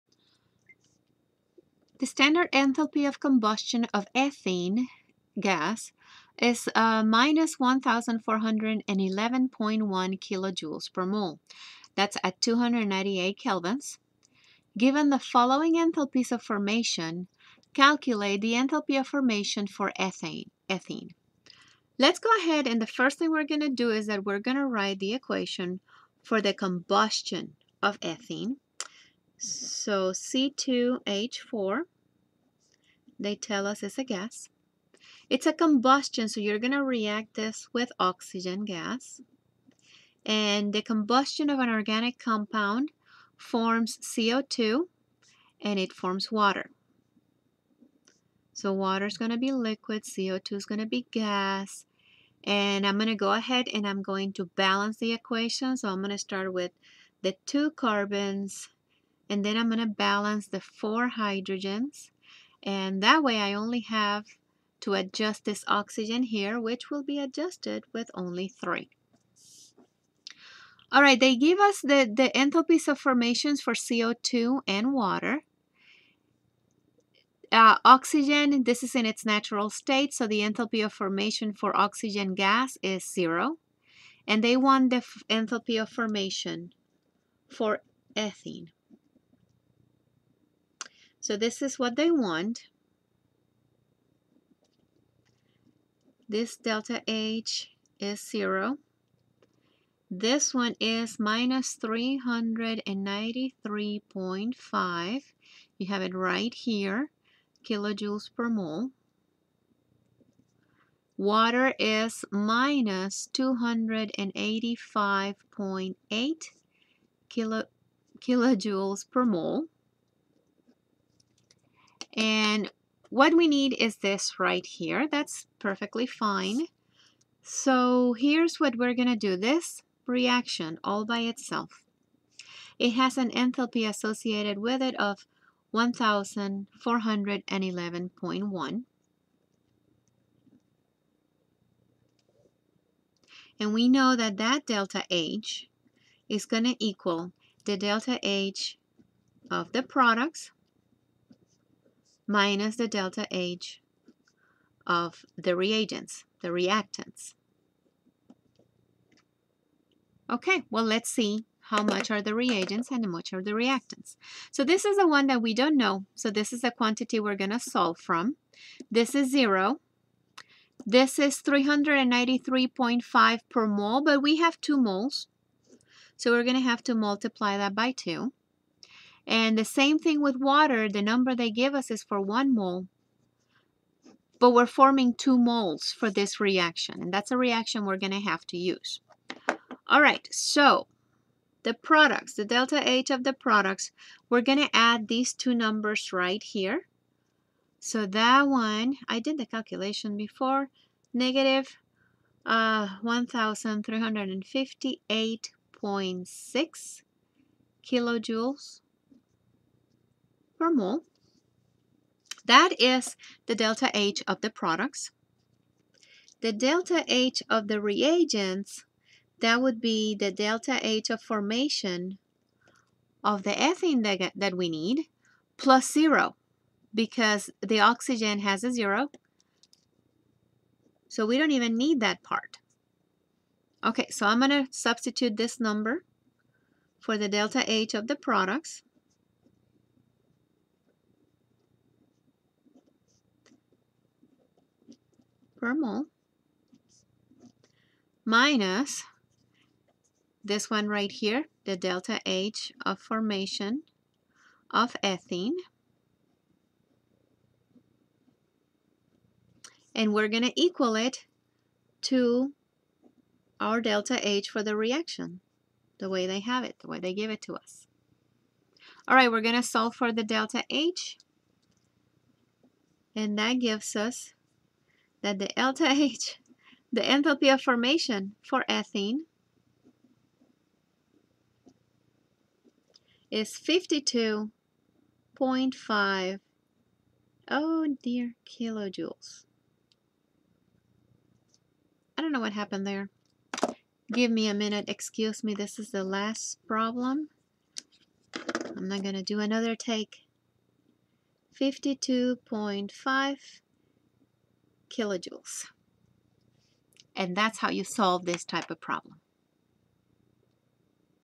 Calculate the enthalphy of formation for C2H4. [DHf CO2 = -393.5 kJ/mol, DHf H2O = -285.8 kJ/mol]
The standard enthalpy of combustion of ethene gas (C2H4) is -1411.1 kJ/mol. Given the following enthalpies of formation, calculate the enthalphy of formation for C2H4. [DHf CO2 = -393.5 kJ/mol, DHf H2O = -285.8 kJ/mol]